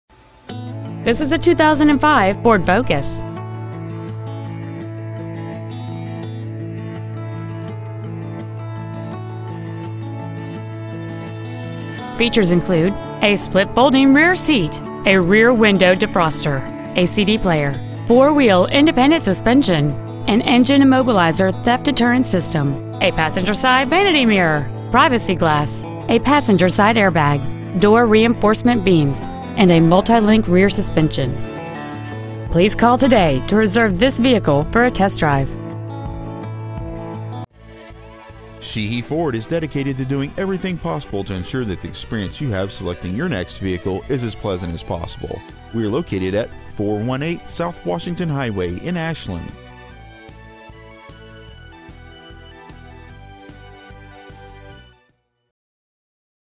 2005 FORD FOCUS Ashland VA
Year : 2005
 Make : FORD
 Model : FOCUS
 Engine : 2.0L I4 16V MPFI DOHC

 Exterior : SILVER
 Miles : 70,060

 Stock : J112767A

 418 S. Washington Hwy.